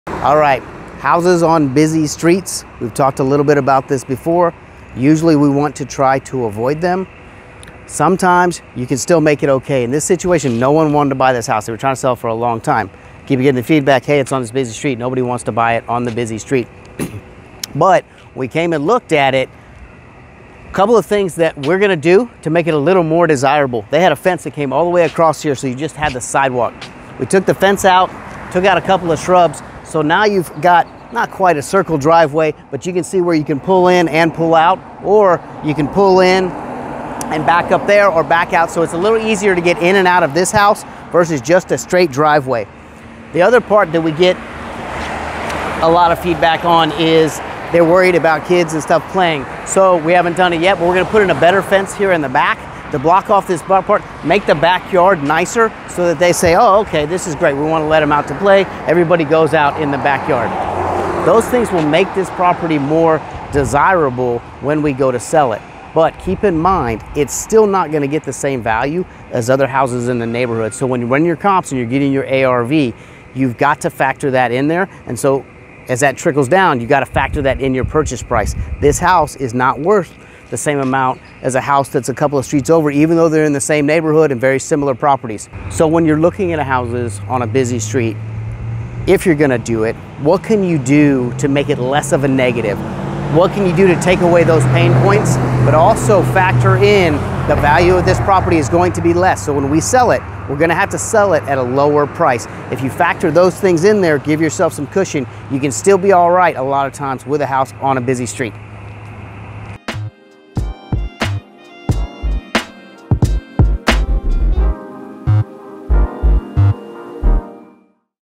Houses On Busy Streets
Houses on busy streets can be tougher to sell. But, if you look for opportunities to remove some of the negatives of a busy street and make sure you price the property appropriately, you can still be successful! Take a look at what we did to make sure this house will be a success even if it is on a busy street!